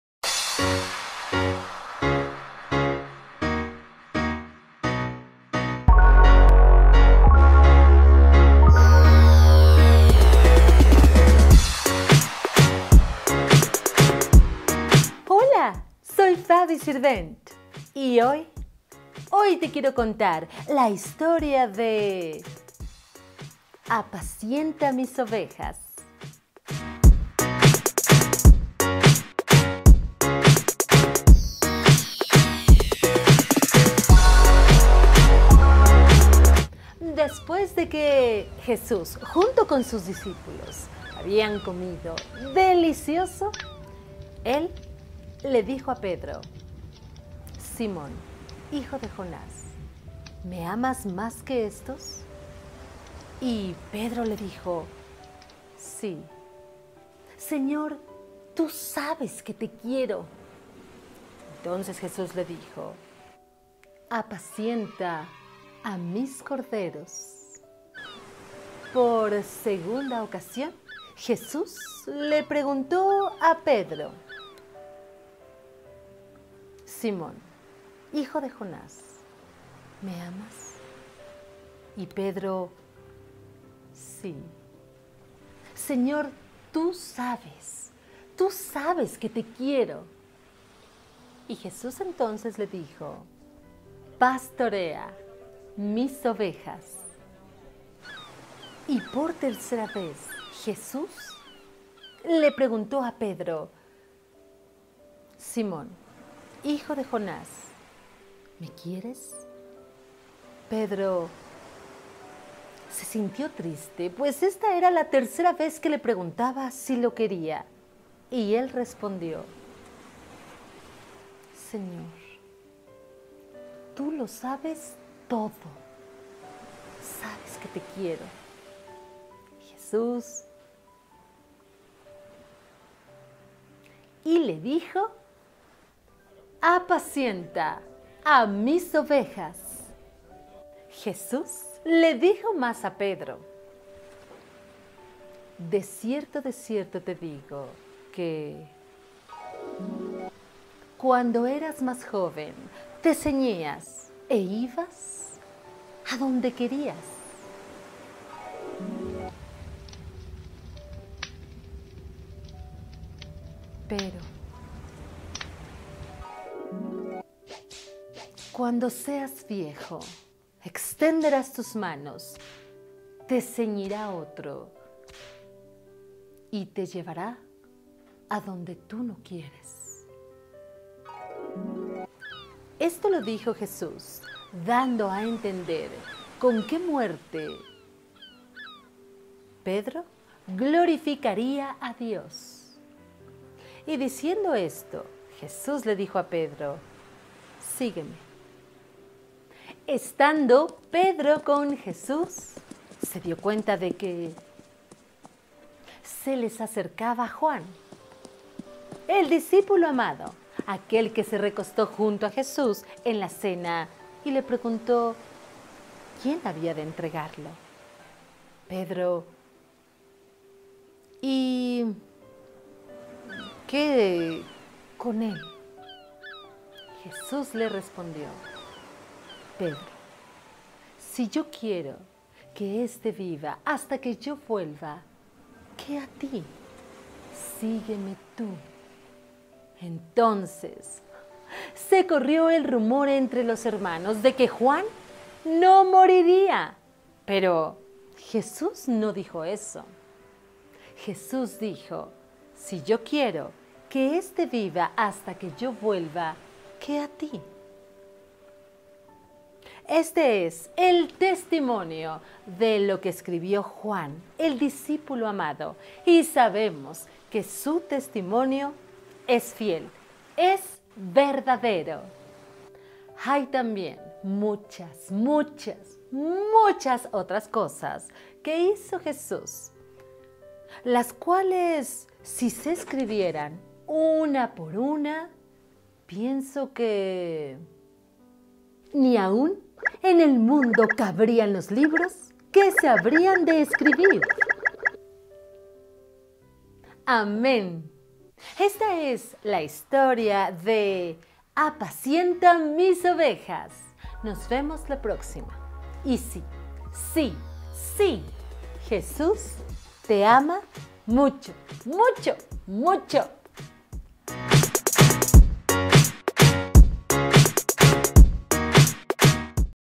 #282 Apacienta a mis ovejas
Hoy te quiero contar... la historia de JESÚS cuando le pide a Pedro cuidar de sus ovejas.

REFERENCIAS

Juan 21:15-25